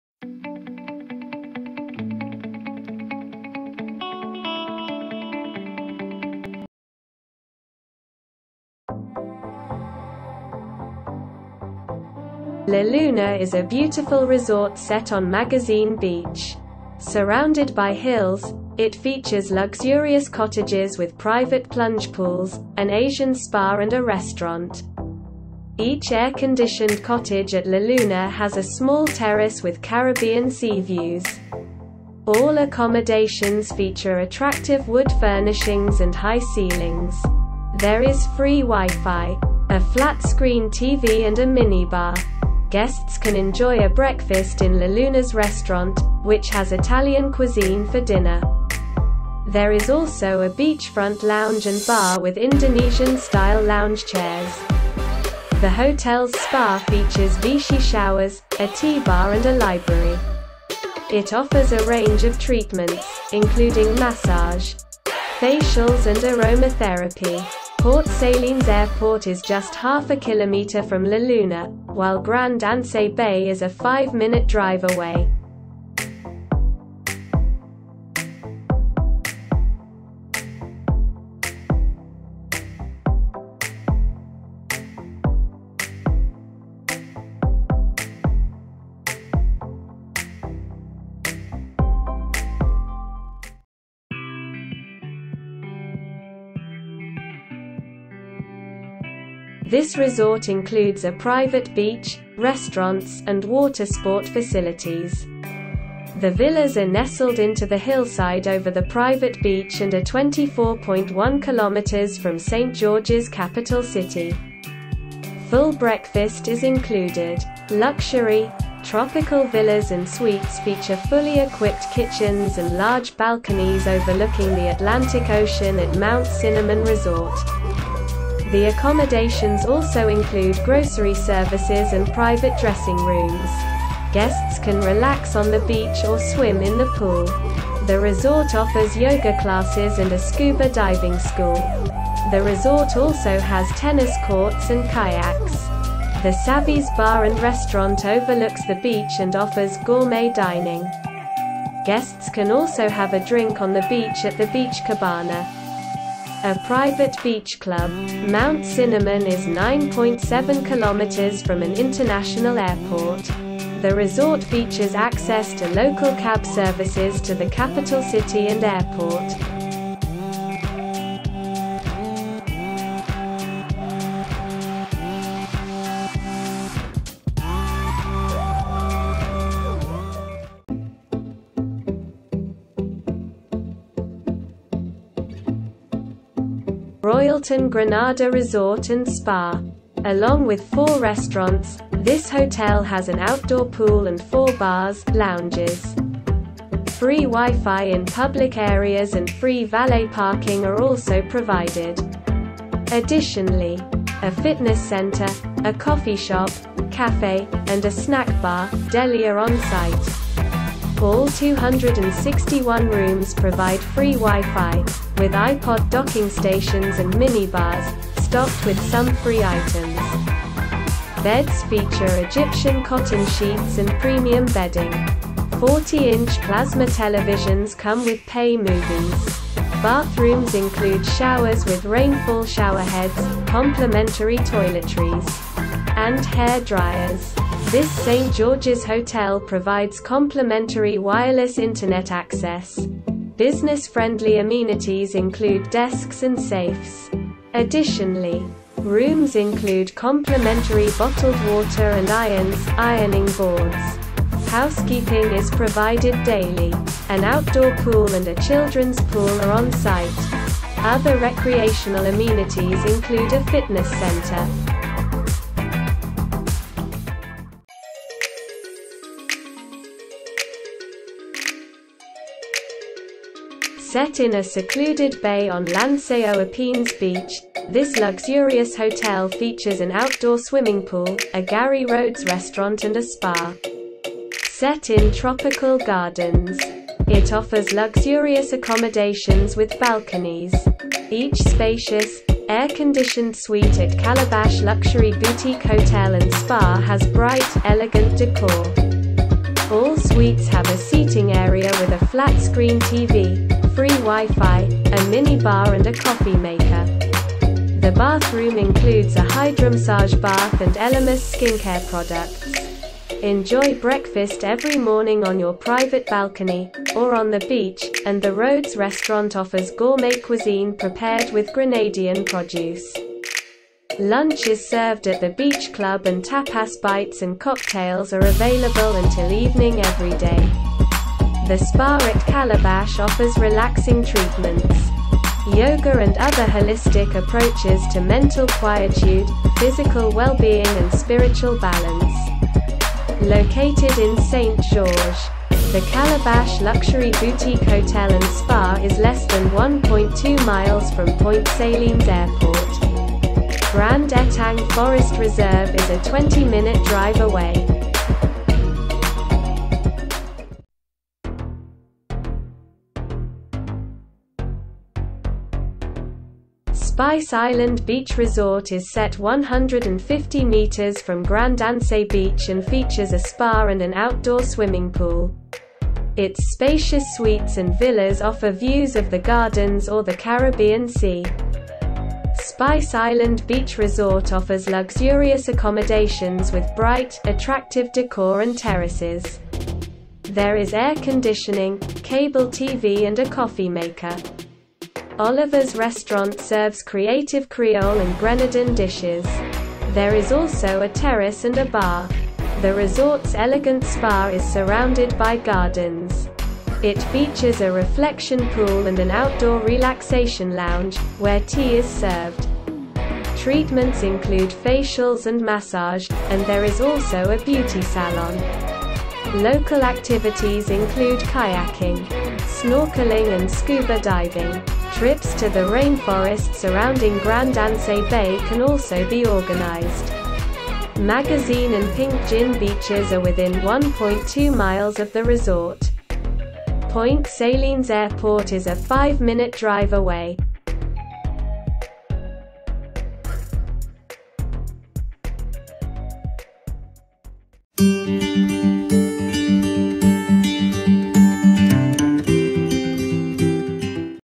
TOP 5 hotels in Grenada, Best Grenada hotels 2021, Grenada
TOP 5 hotels with 5* in Grenada:

1. Spice Island Beach Resort
2. Calabash Luxury Boutique Hotel
 Lance Aux Epines, Grenada Sent Džordžiz, Grenada
3. Royalton Grenada Resort & Spa All inclusive
Magazine Beach Point Selines, St George's, Bamboo, Grenada
4. Mount Cinnamon Resort
Grand Anse Beach, Sent Džordžiz, Grenada
5. Laluna